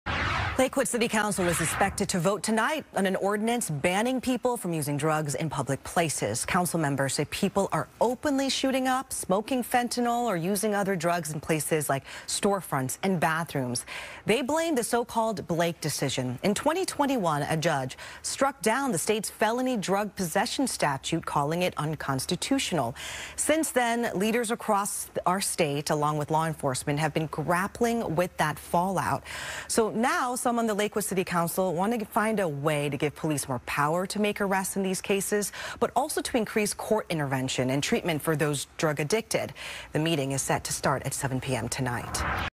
Lakewood to vote on public drug use ban | FOX 13 Seattle
The Lakewood City Council is expected to vote tonight on an ordinance banning people from using drugs in public places.